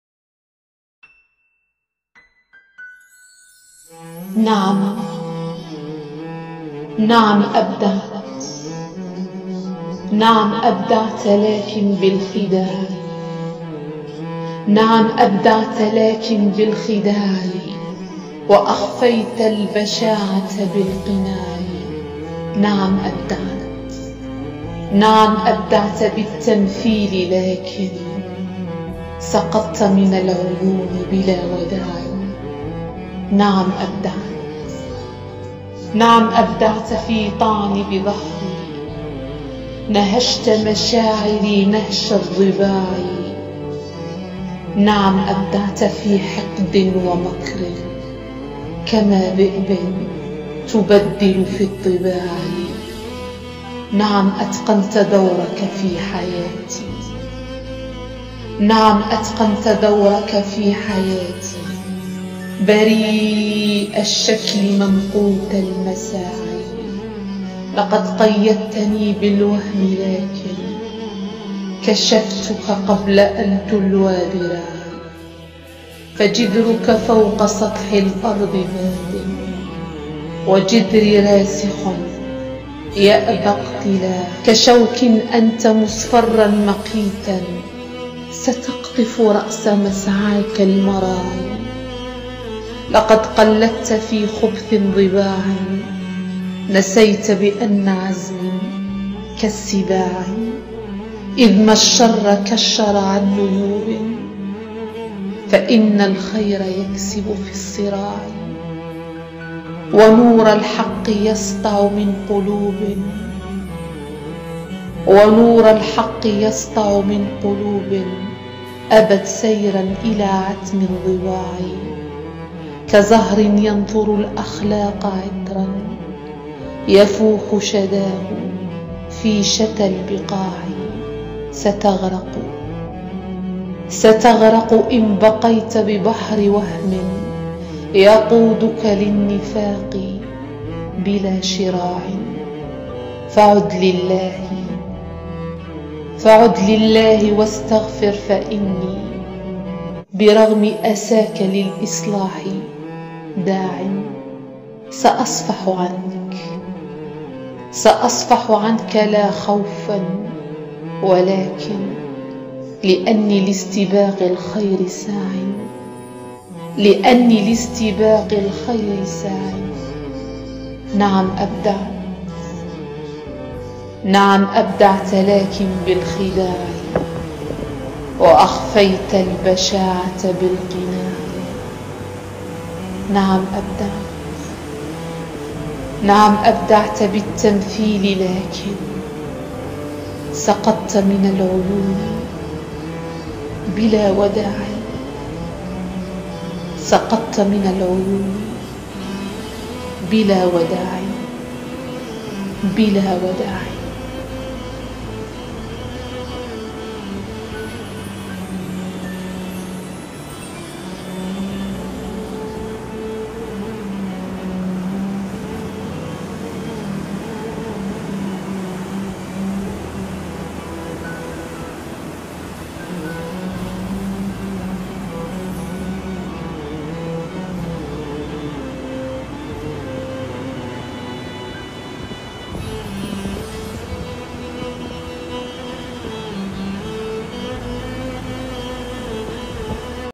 نعم...نعم ابدعت لكن بالخداع د.وائل جحا (رسائل شاعره )
قصيدة نعم ابدعت الدكتور وائل جحا
ألقاء المبدعة غزالة
نعم أبدعتَ لكن بالخداعِ
وأخفيتَ البشاعةَ بالقناعِ
نعم أبدعتَ بالتِّمثيلِ لكن
سقطتَ من العيونِ بلا وداعِ
نعم أبدعتَ في طعني بظهري
نهشتَ مشاعري نهشَ الضِّباعِ
نعم أبدعتَ في حقدٍ ومكرٍ
كما ذئبٍ تُبَدِّلُ في الطِّباعِ
نعم أتقنتَ دورَكَ في حياتي
بريءَ الشَّكلِ ممقوتَ المساعي
لقد قيَّدتني بالوهمِ لكن
كشفتُكَ قبل أن تُلوى ذراعي
فجذرُكَ فوقَ سطحِ الأرضِ بادٍ
وجذري راسخٌ يأبى اقتلاعي
كشوكٍ أنتَ مصفرٍّ مقيتٍ
سَتَقطِفُ رأسَ مسعاكَ المراعي
لقد قلَّدتَ في خبثٍ ضباعًا
نسيتَ بأنَّ عزميَ كالسِّباعِ
إذا ما الشَّرُ كشَّرَ عن نيوبٍ
فإنَّ الخيرَ يكسبُ في الصِّراعِ
ونورُ الحقَّ يسطعُ من قلوبٍ
أبت سيرًا إلى عتم الضياعِ
كزهرٍ ينثرُ الأخلاقَ عطرًا
يفوحُ شذاهُ في شتَّى البقاعِ
ستغرقُ إن بقيتَ ببحر وهمٍ
يقودُكَ للنِّفاقِ بلا شراعِ
فعد للهِ واستغفر فإنِّي
برغم أساكَ للإصلاحِ داعِ
سأصفحُ عنكَ لا خوفًا ولكن
لأنِّي لاستباقِ الخيرِ ساعِ
وائل جحا
مواقع التواصل الاجتماعي
 الفايسبوك

شعر قصائد د_وائل_ جحا